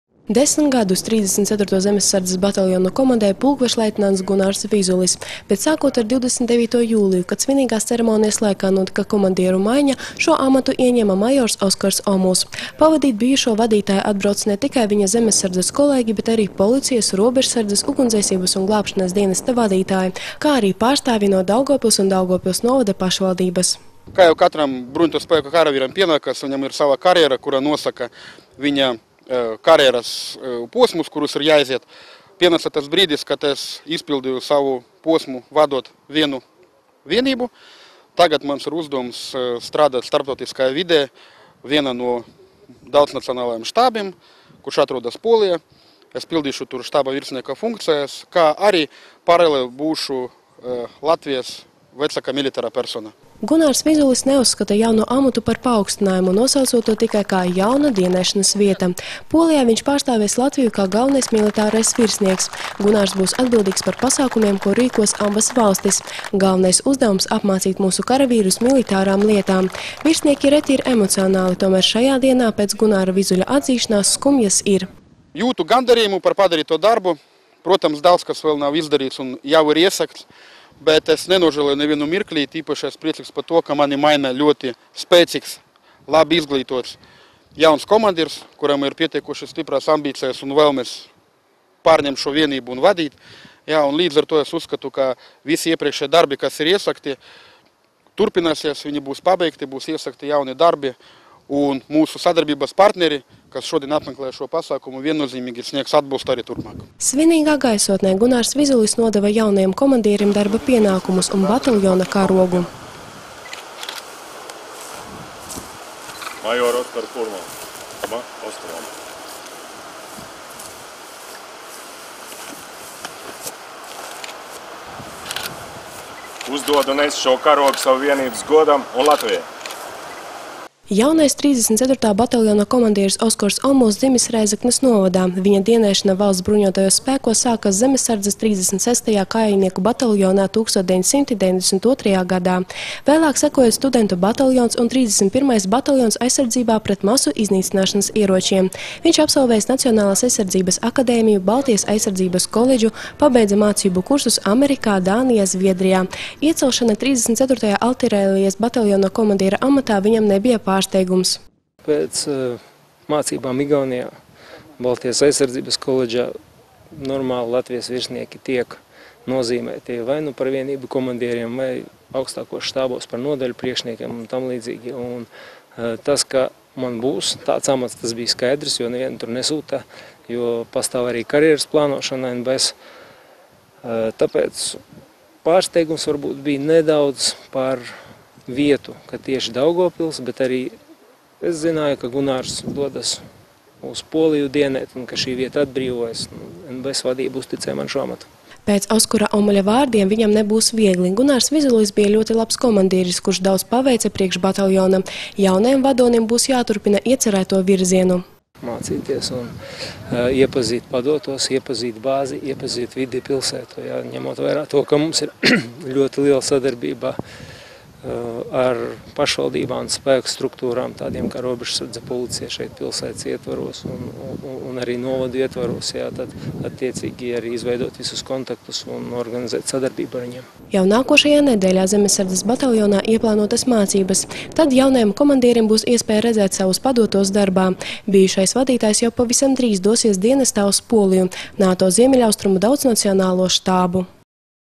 Zemessardzes 34.Artilērijas bataljonam jauns komandieris
Trešdien, 29.jūlijā, Daugavpilī līdzšinējais komandieris pulkvežleitnants Gunārs Vizulis svinīgi nodeva amata pienākumus un bataljona karogu jaunajam komandierim majoram Oskaram Omuļam.